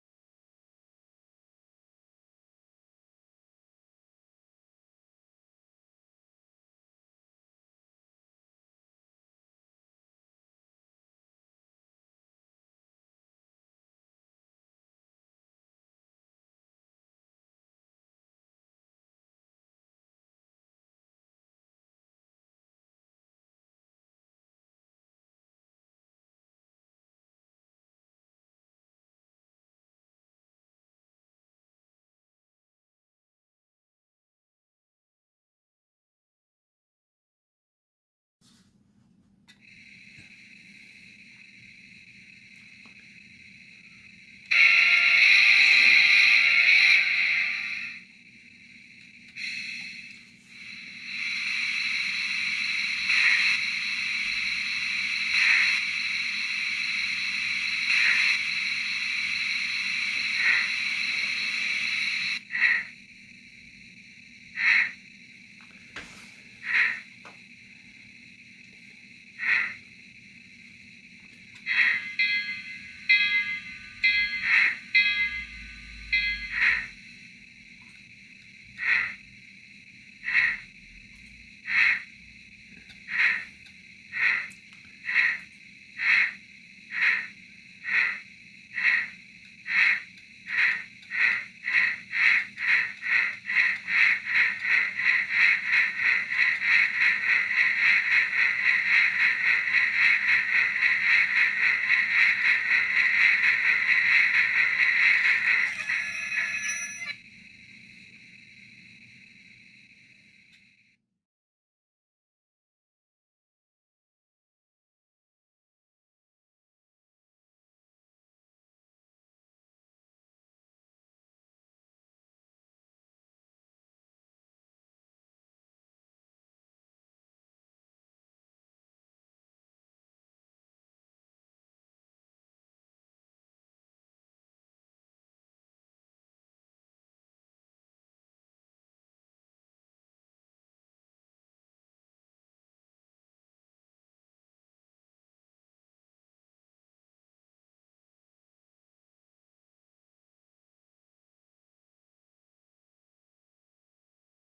Yamix Test Stand - First Look
Took 6 weeks to arrive, and is simplicity itself!   Every unit, steam or diesel, I've placed on it has run first time, with no issues. Do need to fiddle to get all wheels / drivers seated correctly, but hey, this is N scale!  And, as can be seen in the video, easy to adjust the rollers.

Caution:  Do not put a roller beneath any axle or driver with a traction tire - your loco will then move off the rollers!